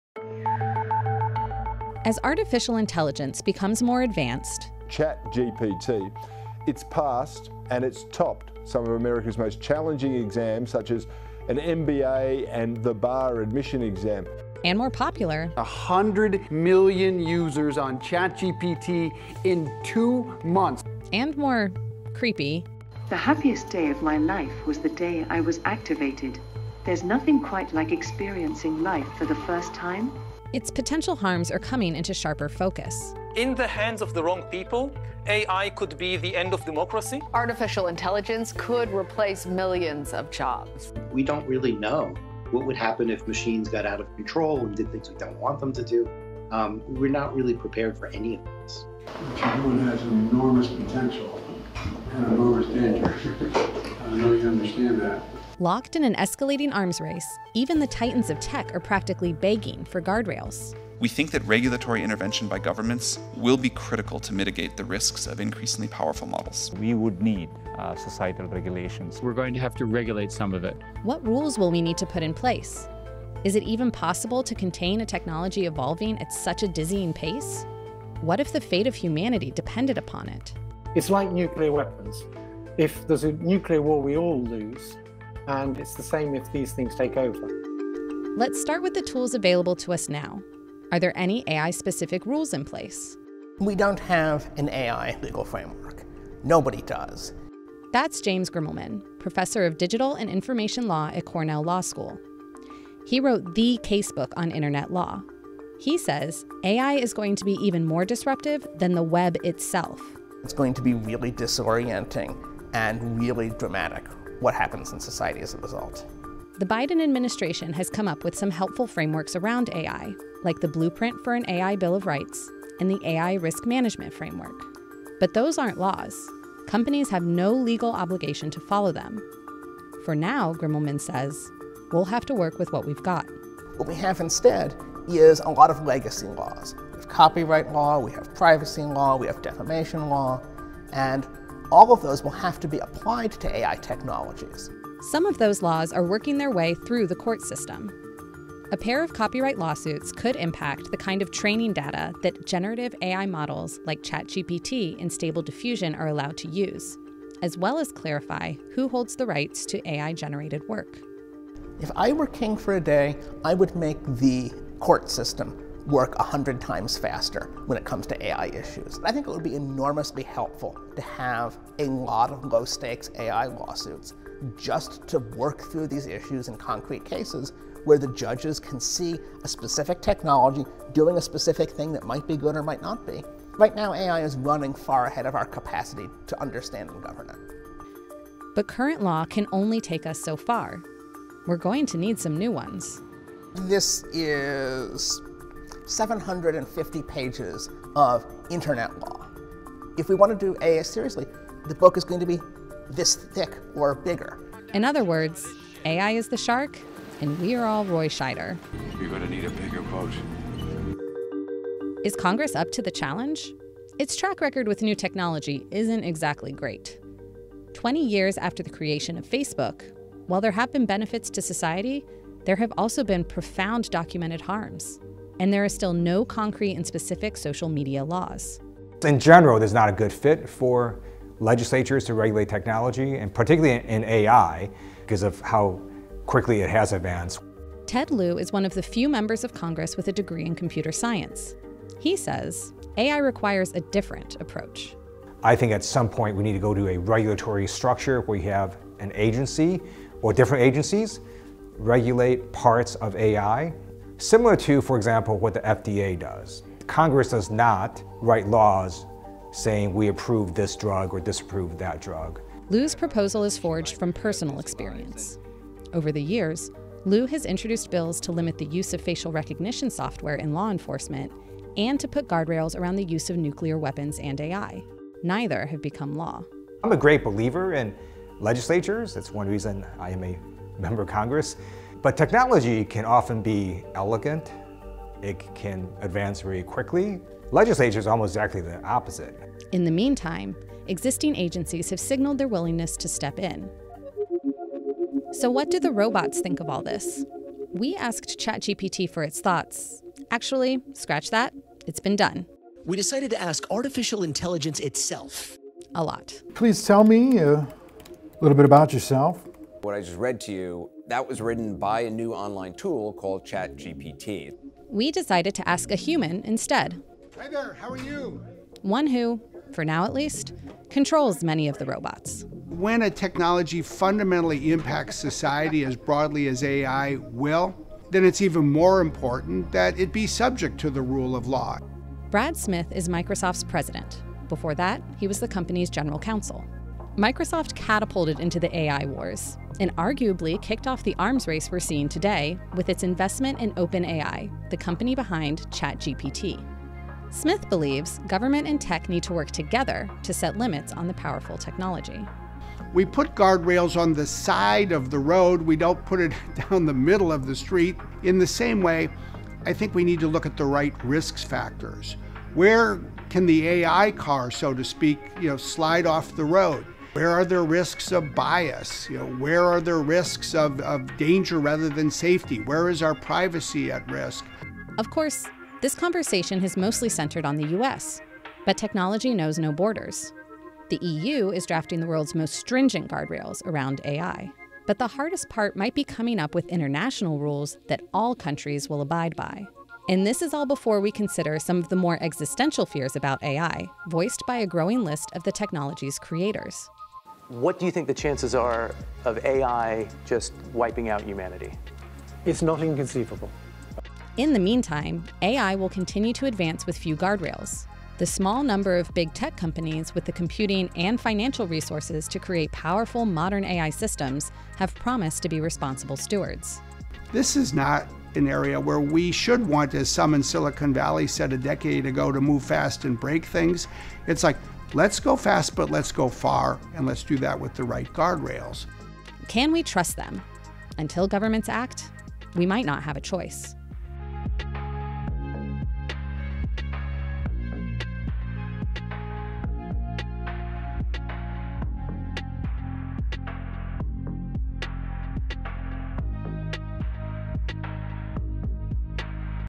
The Artificial Intelligence Dilemma: Can Laws Keep Up?
On May 16, a Senate subcommittee held its first congressional hearing on artificial intelligence. While the tech industry touts AI's many benefits - personalized learning, content creation, scientific discovery - the technology's risks have also come into focus: disinformation, job loss, and, some experts warn, AI could be an existential threat to humanity. Now Congress is paying attention. But the question of how, or even if the government is capable of putting guardrails around such a fast-moving technology, is unclear.

In this video, we look at what laws are currently in place to regulate AI, what options lawmakers should consider, and what role the tech industry should play.

Video features:
Rep. Ted Lieu (D-CA)
Brad Smith, President, Microsoft
James Grimmelmann, Cornell Law School professor of tech and IP law